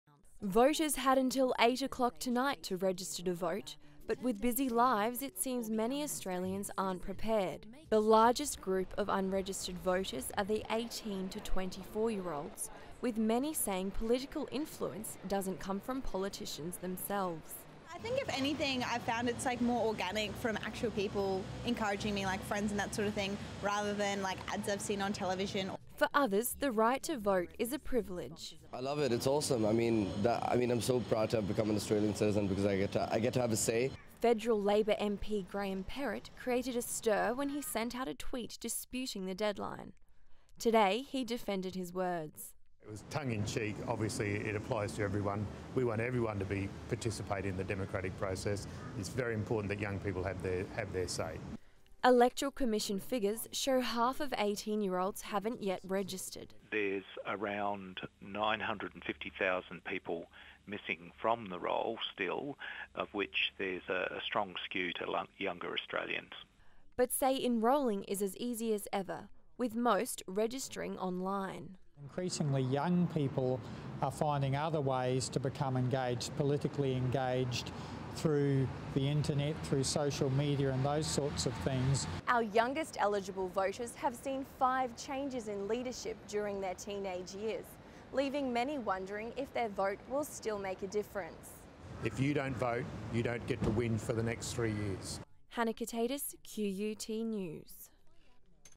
Last minute rush to enroll
Almost one million people are missing out on their chance to vote. With the election looking like it'll come down to the wire, time is running out to have your say.

Hannah Kotaidis reports